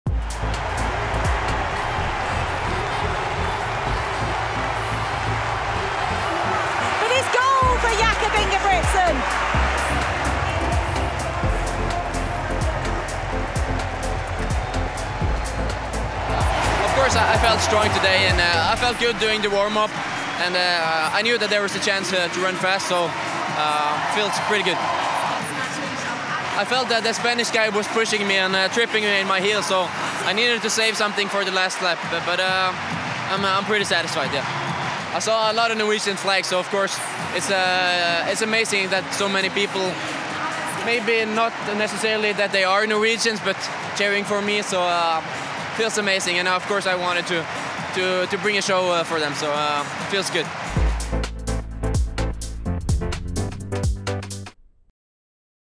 Jakob Ingebrigtsen's double double with 1500m gold - European Athletics Championships – Munich 2022
Norway’s king of the track was at it again on Thursday, roared on by a packed crowd in the Munich Olympic stadium, winning 1500m gold.
After his 5,000m golden show 48 hours before, Jakob Ingebrigtsen was in cruise-control around the four laps, storming to a 'double double' glory.

Not content with a mighty medal haul, Ingebrigtsen, still only 21, also smashed the championship record, clocking 3 mins 32.76 secs.